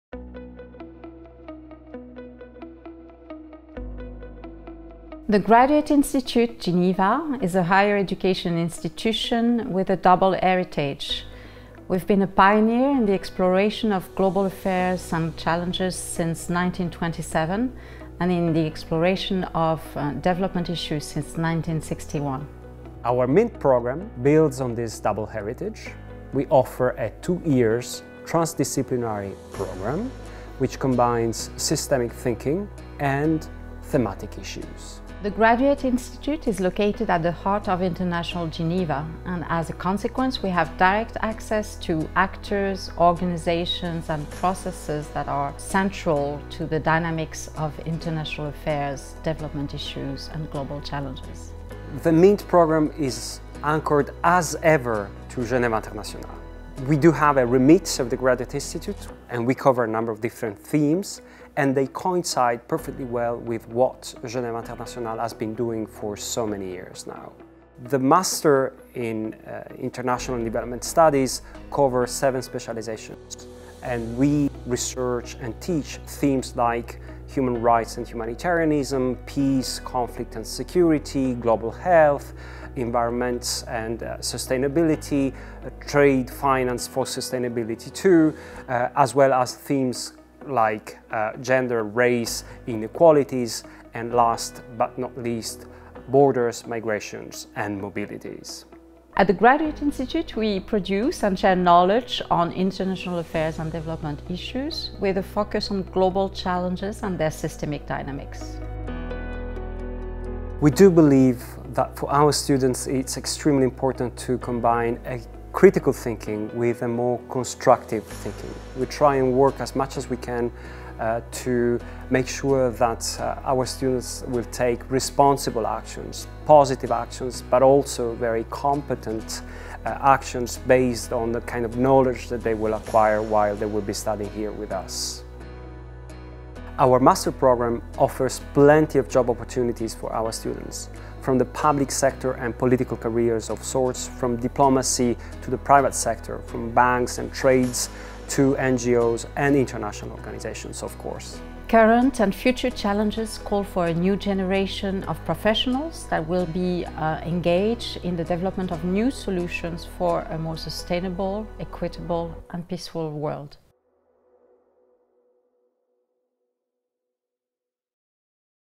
MINT Programme at the Graduate Institute
The new Master in International and Development Studies is anchored as strongly as ever to Genève internationale. The Graduate Institute's research and teaching remit coincide with what international Geneva has been standing for more than one century.